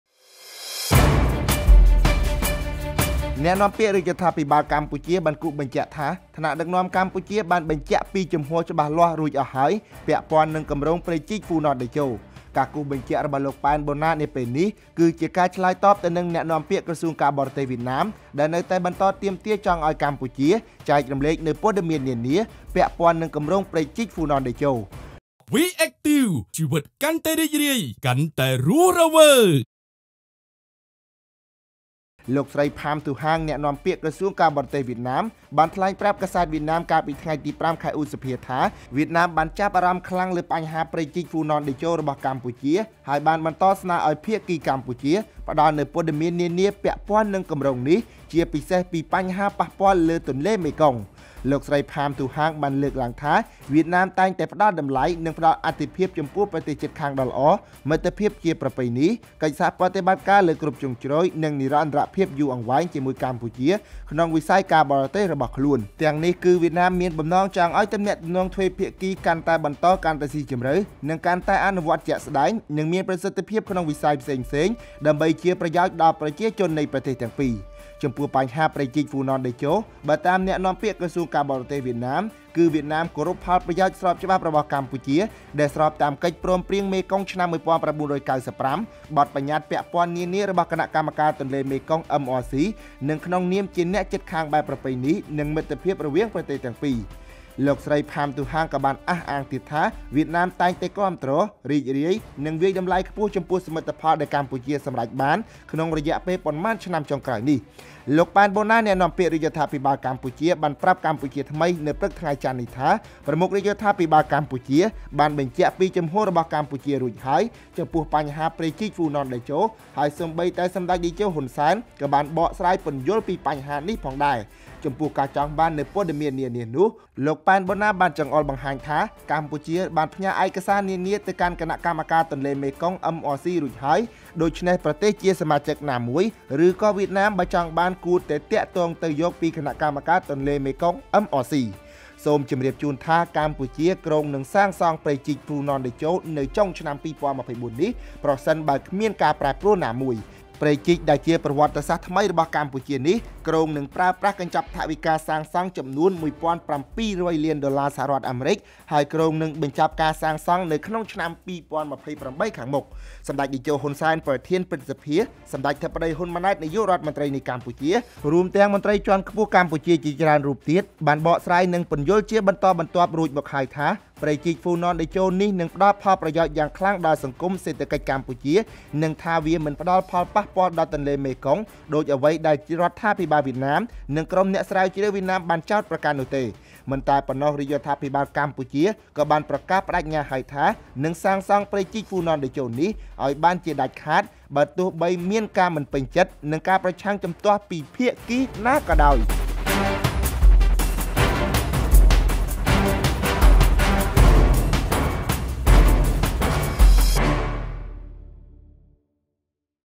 លោក ប៉ែន បូណា៖ ថ្នាក់ដឹកនាំកម្ពុជា បានបញ្ជាក់​ជំហរច្បាស់លាស់អស់ហើយចំពោះបញ្ហាព្រែកជីកហ្វូណនតេជោ
អ្នកនាំពាក្យរាជរដ្ឋាភិបាលកម្ពុជា លោក ប៉ែន បូណា បានគូសបញ្ជាក់ថា ថ្នាក់ដឹកនាំកម្ពុជា បានកញ្ជាក់ពីជំហរច្បាស់លាស់រួចអស់ហើយពាក់ព័ន្ធនឹងគម្រោងព្រែកជីកហ្វូណនតេជោនេះ។ ការគូសបញ្ជាក់របស់លោក ប៉ែន បូណា នេះ គឺជាការឆ្លើយតបទៅនឹងអ្នកនាំពាក្យក្រសួងការបរទេសវៀតណាមដែលនៅតែបន្តទាមទារចង់ឱ្យកម្ពុជាចែករំលែកនូវព័ត៌មាននានាពាក់ព័ន្ធនឹងគម្រោងព្រែកជីកហ្វូណនតេជោ